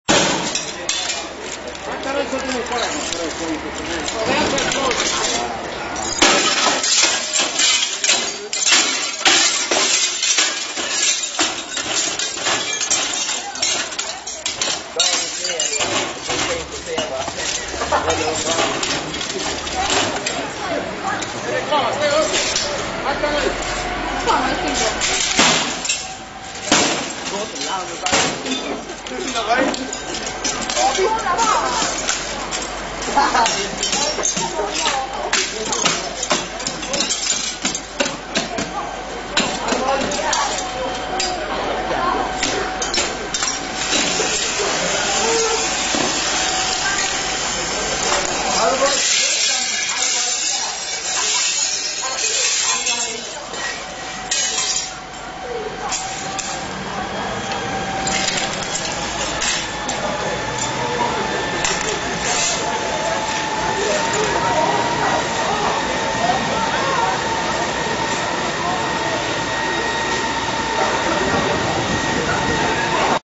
Räddningstjänsten möttes av kraftig brand
Brandmännen möttes av en kraftig brand när de forcerat ingången till butiken i centrala Helsingborg.

Publicerad på hd.se söndag 13 augusti 2006.